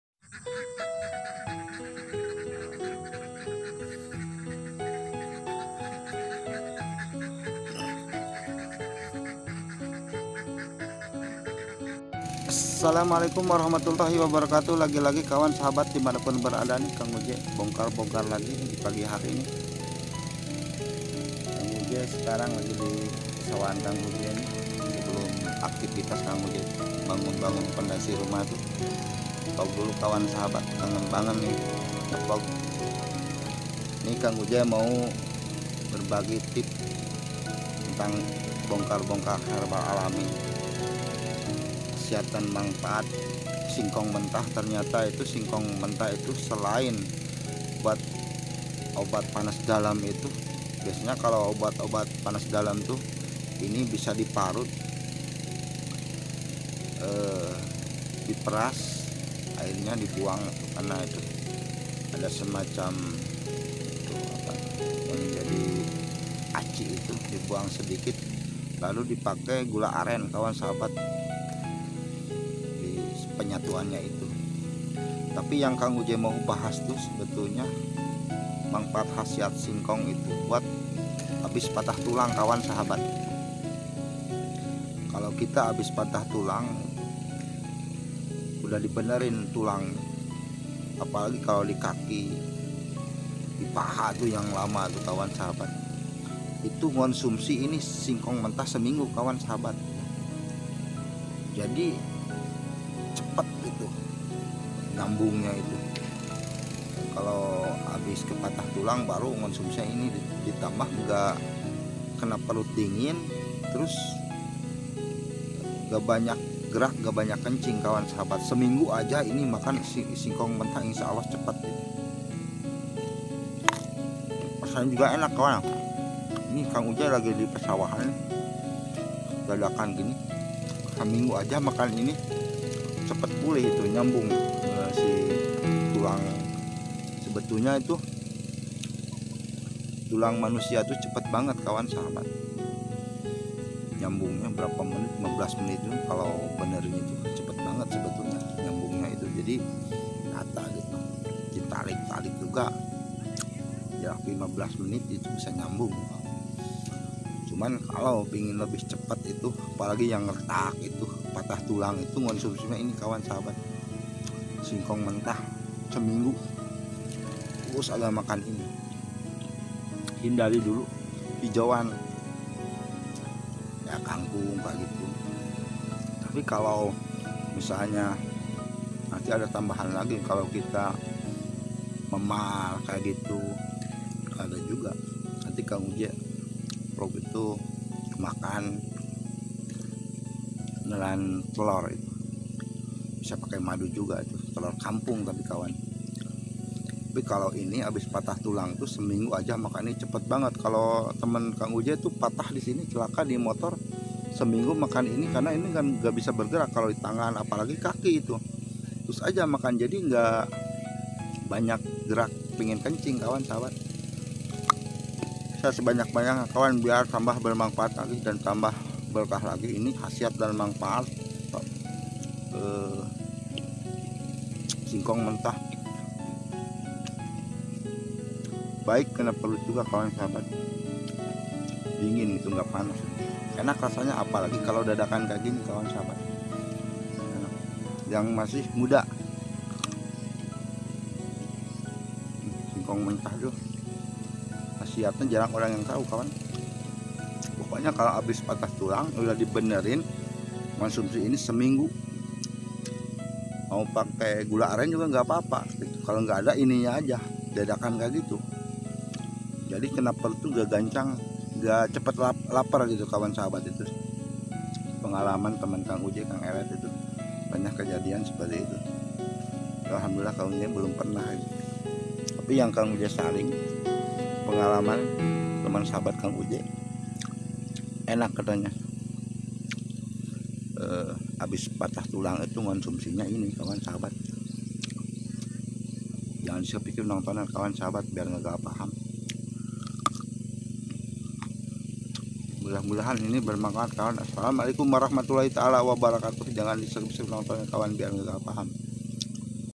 LUAR BIASA manfaat SINGKONG MENTAH
ambil yg masih muda sehabis patah tulang udah d benerin ke ahli tulang makan singkong mentah seminggu ajah sembuh total cepet pulihyh, mau tambah gula aren juga bolah,patah tulang#retak tulang#memar#